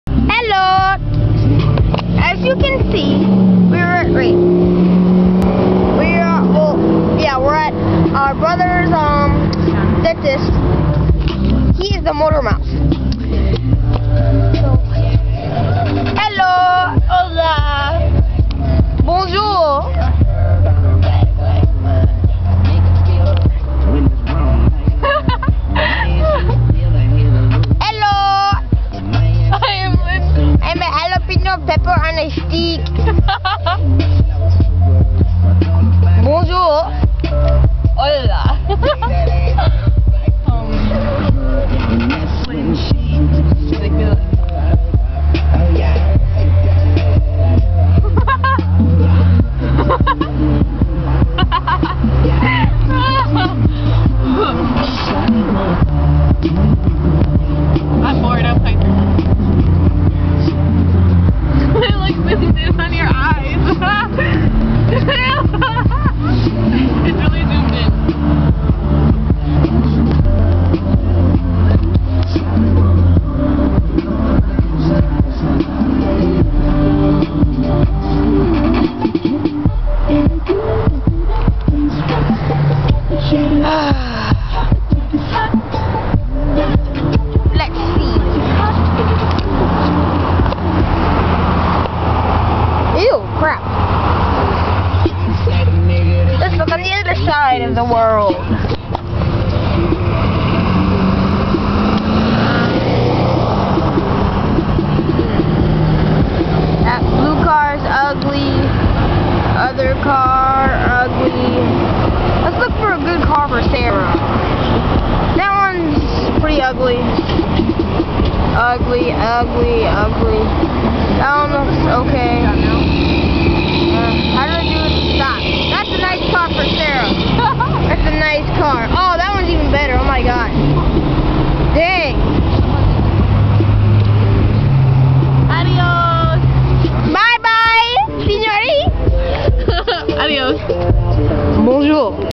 Car Fun With Shawn
sitting in the car with my youngest brother waiting for our other brother to come out of his ortho apt.